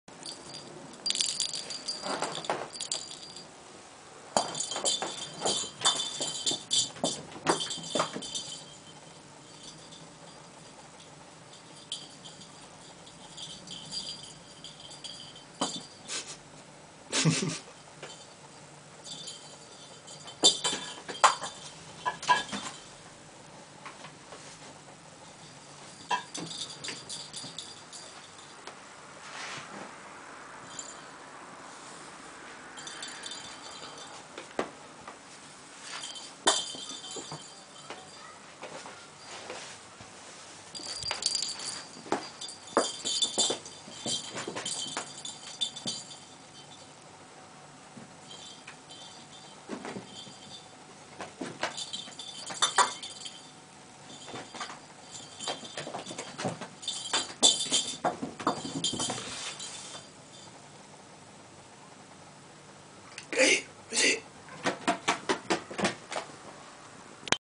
The kitten plays fetch.
"kitten" (she's actually 3 months old now) brings the ball back